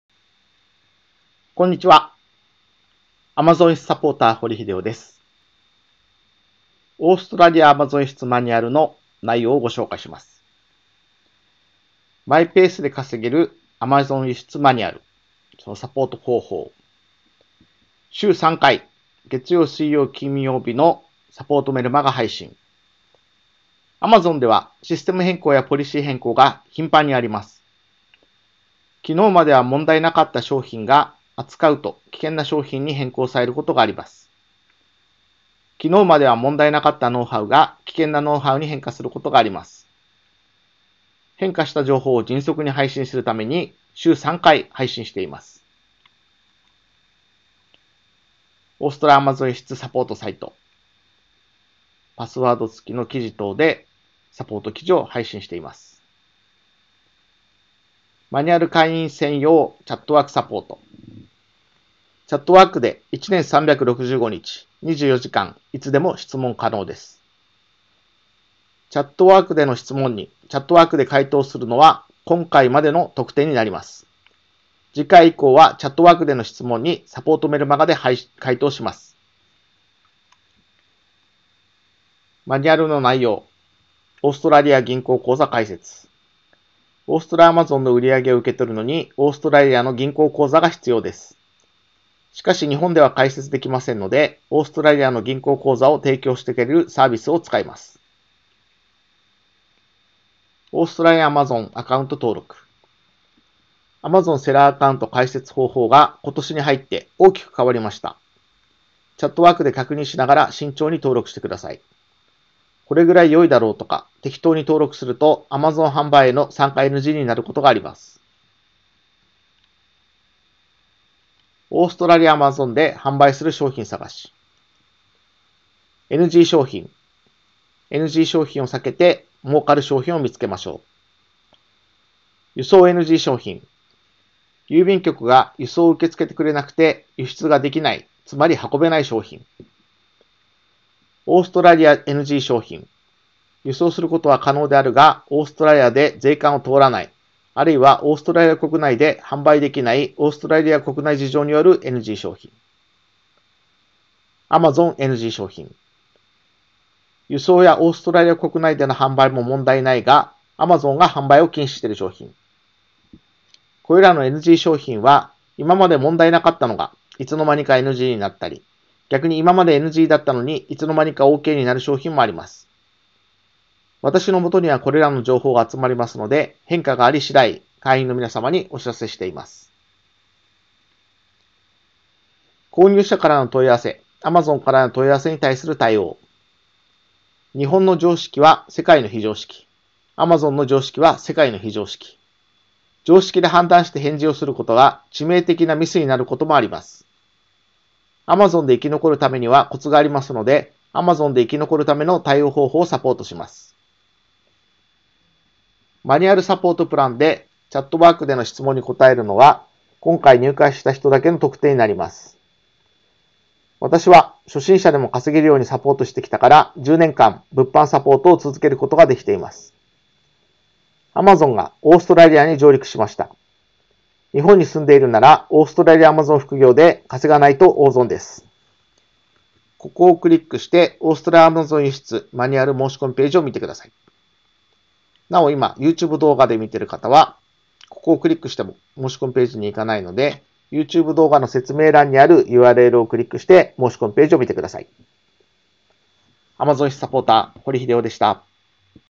アマゾンで生き残るためにはコツがあります
オーストラリアAmazon輸出マニュアル内容
アマゾンで生き残るための対応方法をサポートします。
私は初心者でも稼げるようにサポートしてきたから10年間物販サポートを続けることができています。

この動画は大人の方が輸出ビジネスに取り組む際に参考になるように作成した動画になります。
13歳以下の方がご視聴される際は保護者の方とご一緒にご覧ください。

1日10分で月10万円を稼ぐ! Amazon輸出ビジネスがわかる本(2013/11/22)